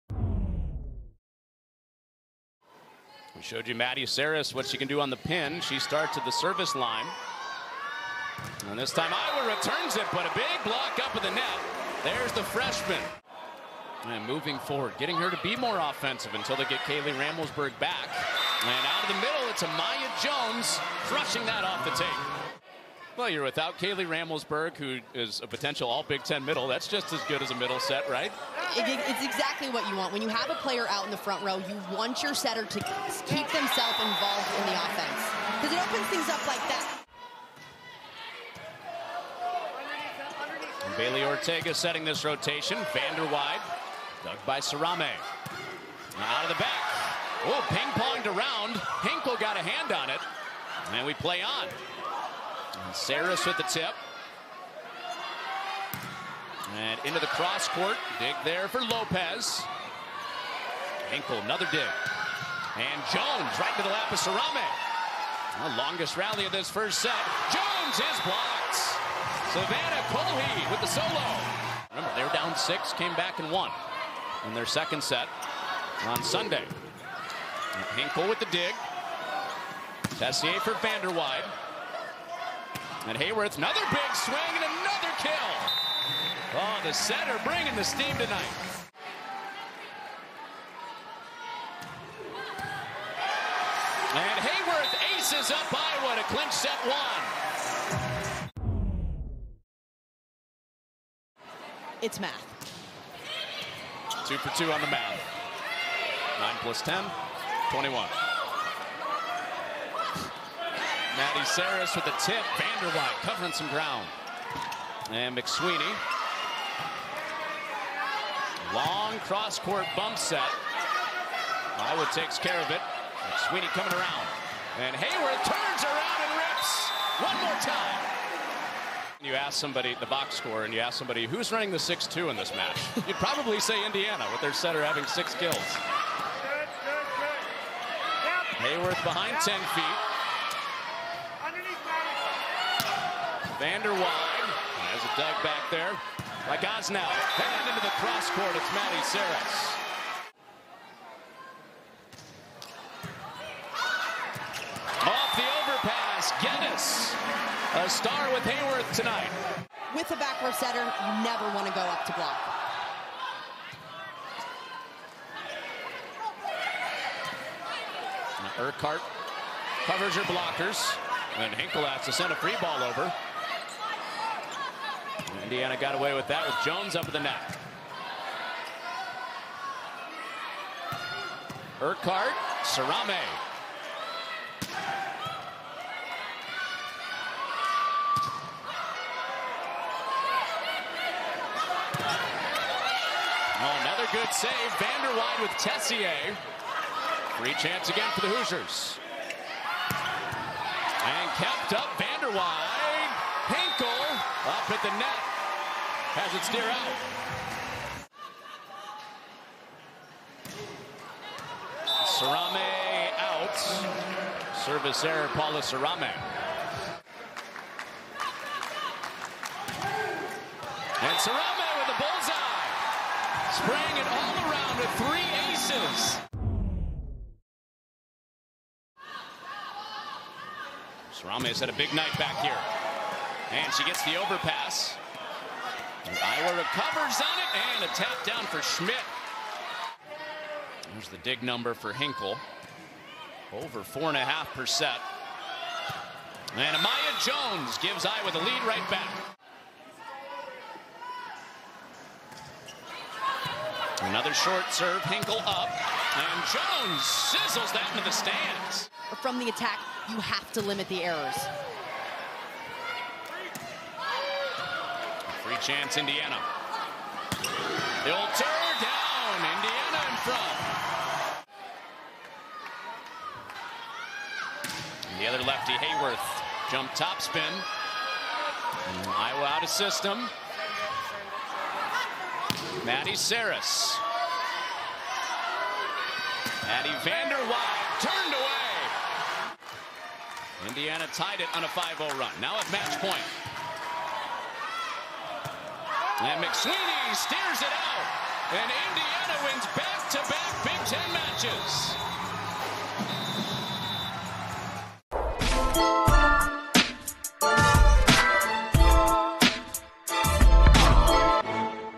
Iowa at Indiana | Highlights | Big Ten Volleyball | Sept. 29, 2022
The Indiana Hoosiers swept the Iowa Hawkeyes in three sets.

00:00:00 -
00:00:03 - 1st Set
00:01:41 - 2nd Set
00:03:49 - 3rd Set